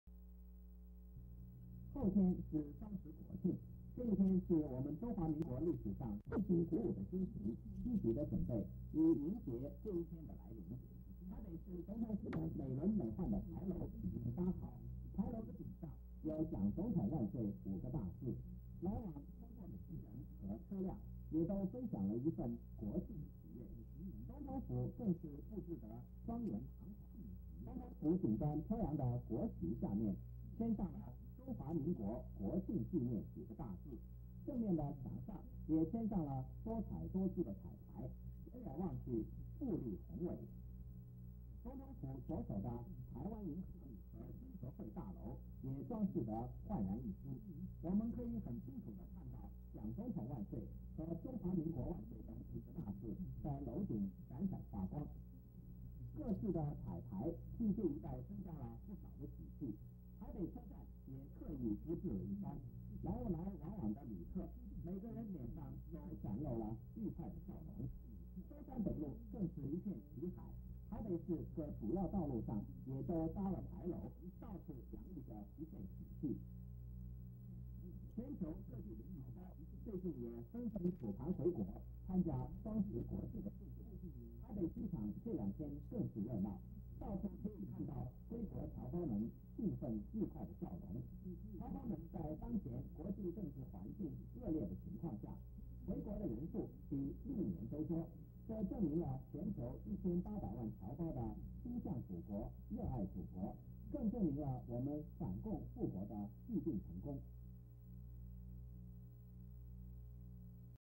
國慶前北市街頭
(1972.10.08)國慶前台北市街頭氣氛.